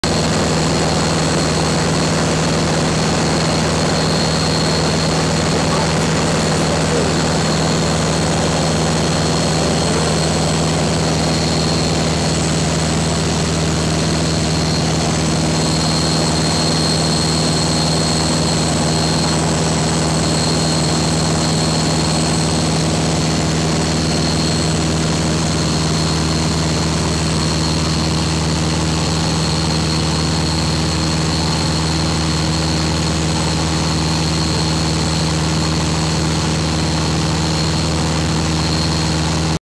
Flying near Biloxi at night
A night flight on the way to New Orleans.. passing just north of Biloxi, MS. level at 4,500 ft.